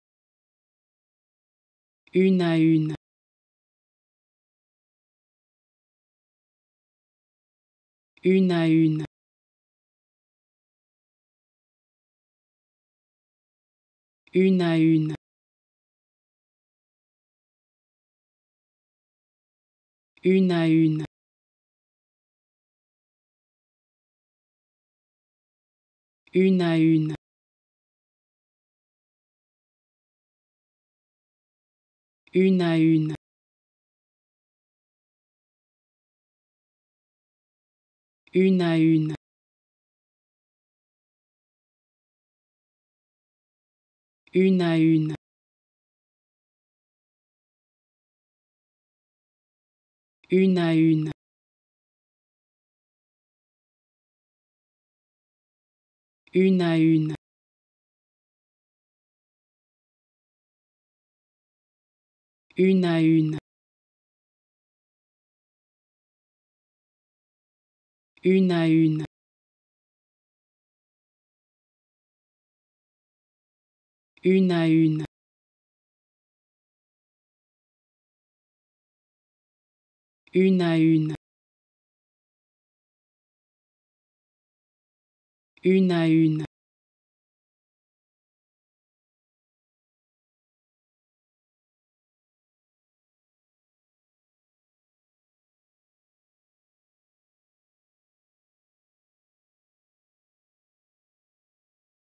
How to Pronounce ''Une à une'' (One by one) Correctly in French
Learn how to say and properly pronounce ''Une à une'' in French with this free pronunciation tutorial.